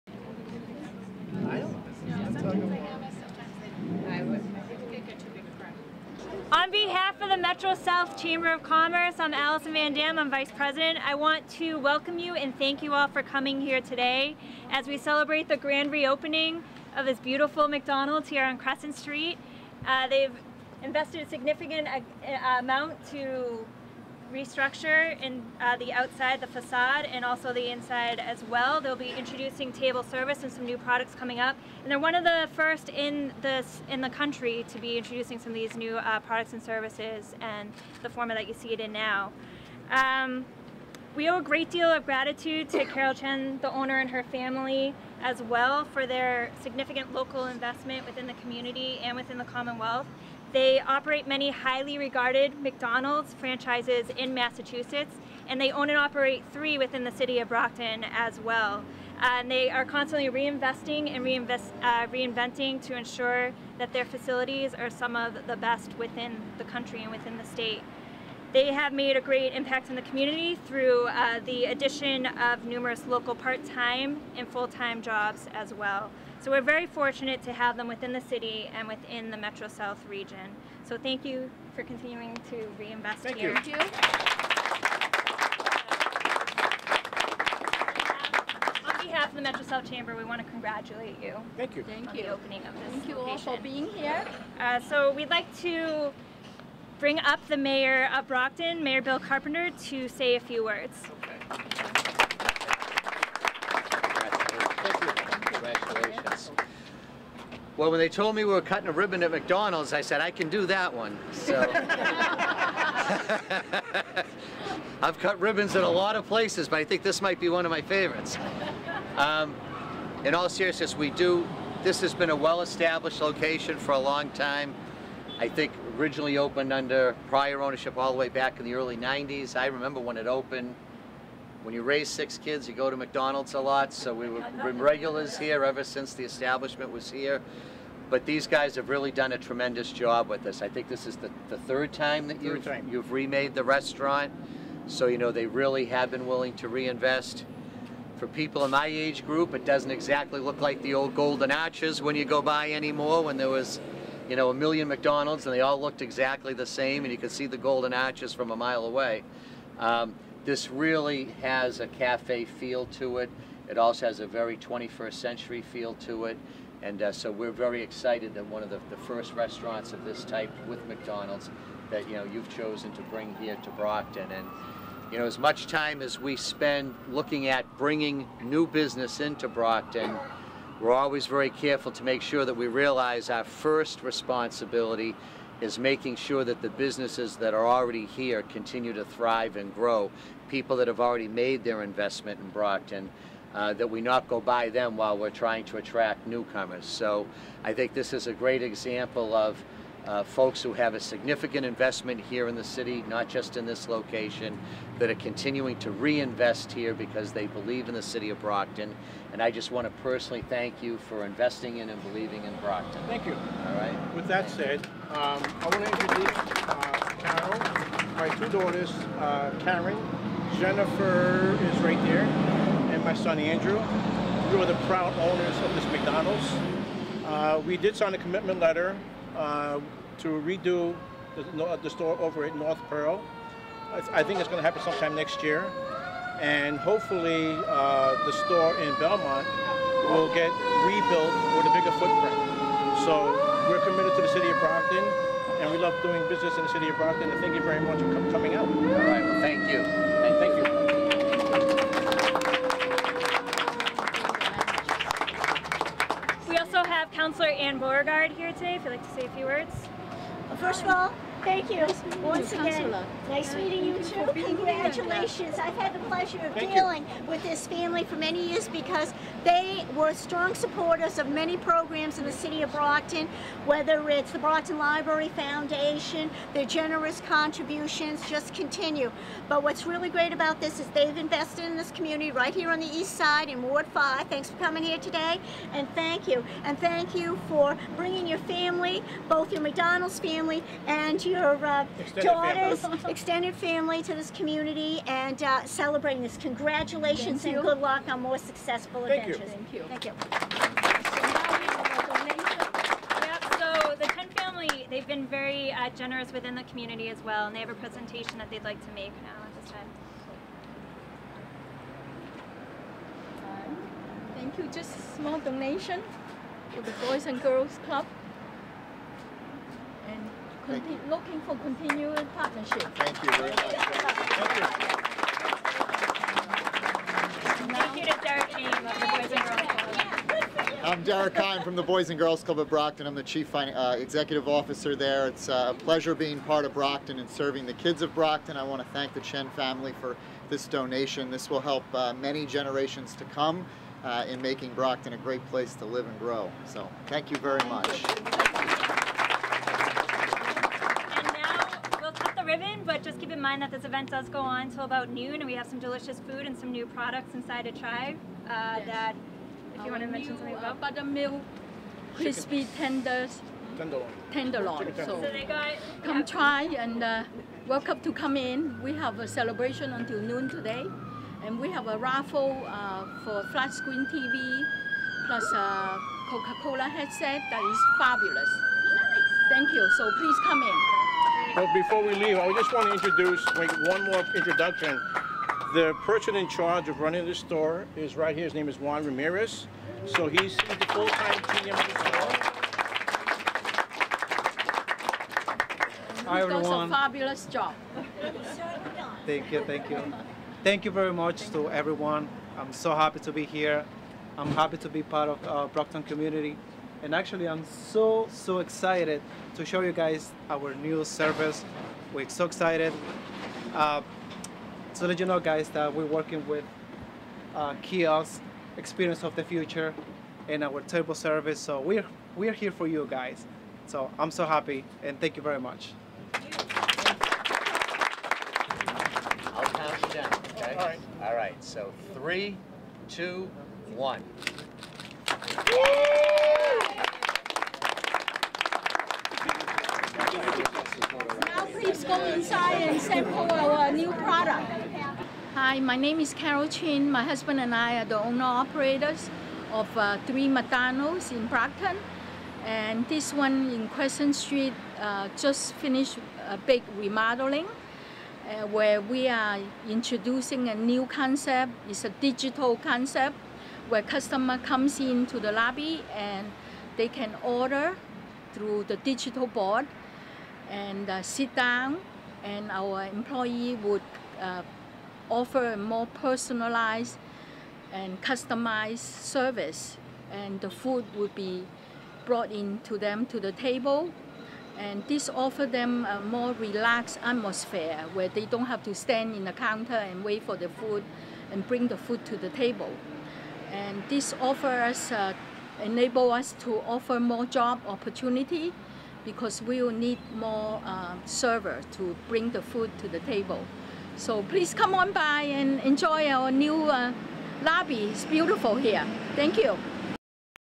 McDonald's Ribbon Cutting Ceremony 9-18-17
McDonald's on Crescent Street in Brockton had their Ribbon Cutting Ceremony